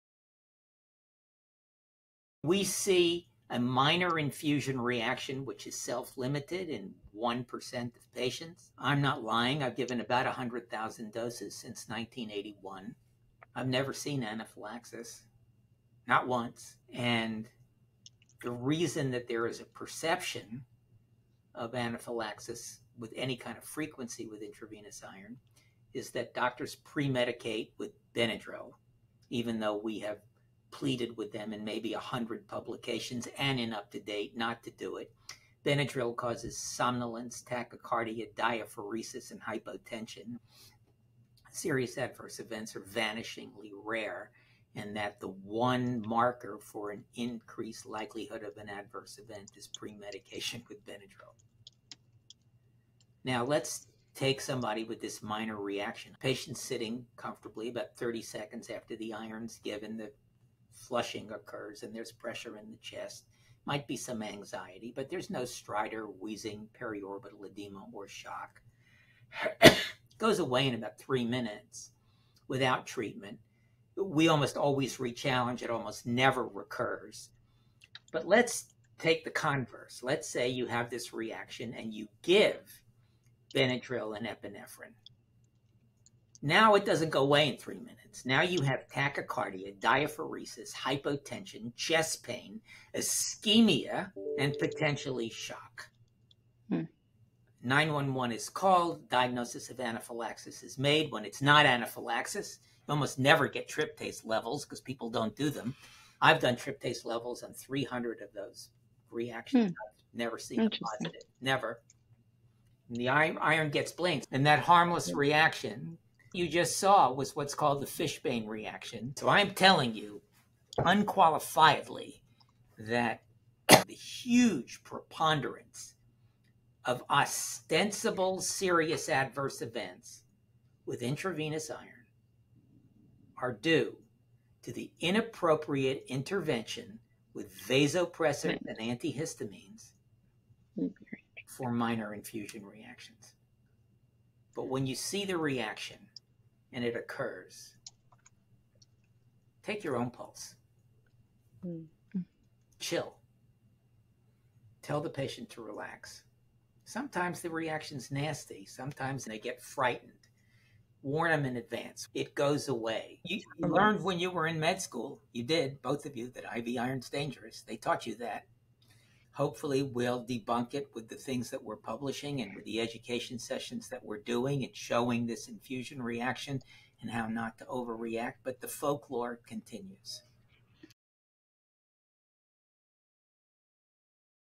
IV Iron Infusion Reactions
Concerned your patient might have an infusion reaction or allergy? Expert hematologist and UpToDate author on the topic, Dr. Michael Auerbach shares his insight about how to appropriately diagnose and manage allergic vs pseudo-allergic reactions in patients receiving IV iron.

Key Points from this video:
-A reaction is almost always a "Fishbane Reaction" or complement activation-related pseudo-allergy (CARPA)
-Typical symptoms: chest pressure, flushing, anxiety which subside when the infusion is held
- Benadryl is NOT recommended either as premedication or in response to an infusion reaction
- Anaphylaxis is exceedingly rare

Original interview by Shreya Trivedi MD and Maria J. Fernandez Turizo MD.
Video content and editing by Alice Kennedy MD.